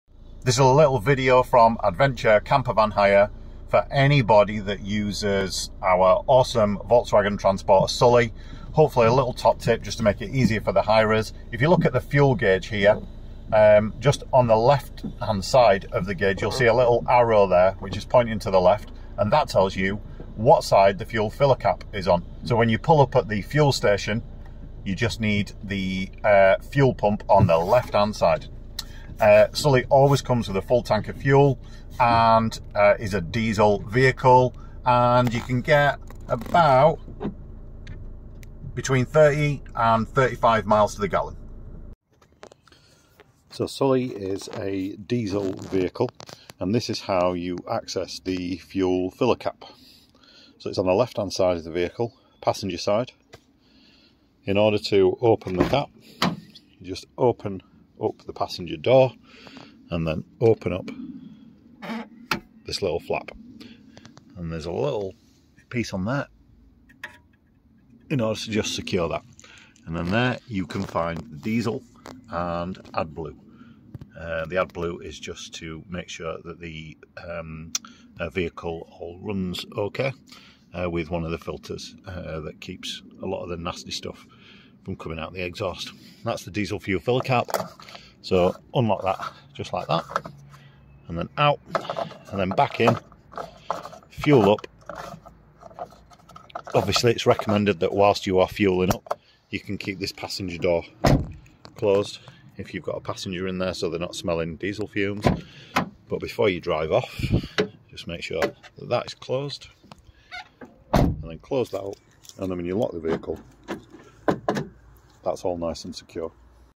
A few useful bits of information for hirers of our Volkswagen Campervan called 'Sully!'
What side of the vehicle is our Volkswagen Transporter Campervan Sully's fuel filler cap? What fuel does it take? How many miles to the gallon? How do I access the fuel filler cap?
Hope this video helps all of Sully's future hirers! Sully is also a euro 6 diesel engine!